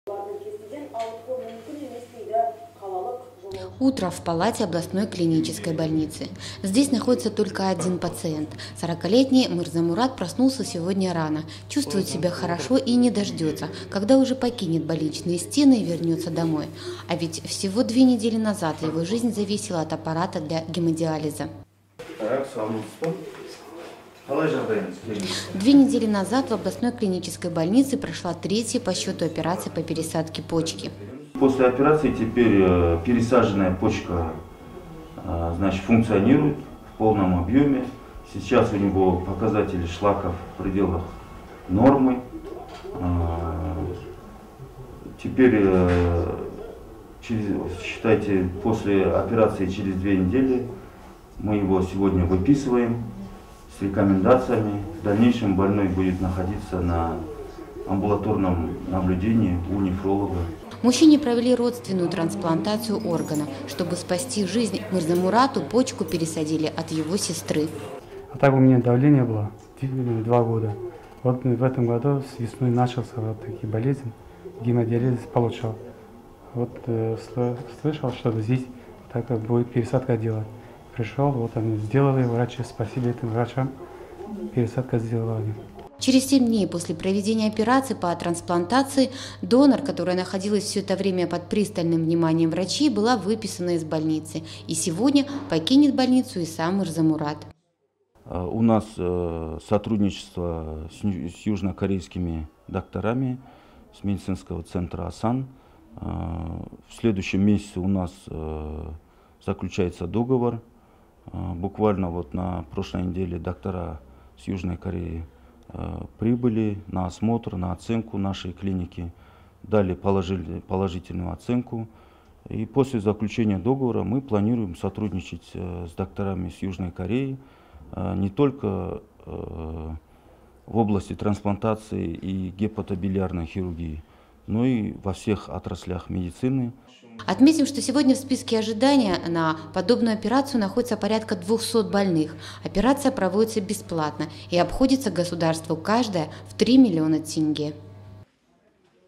Врачи ОКБ ЮКО провели третью операцию по пересадке почки
Третью операция по трансплантации почки провели врачи областной больницы скорой медицинской помощи. Спустя две недели, пациент чувствует себя хорошо - все показатели в норме.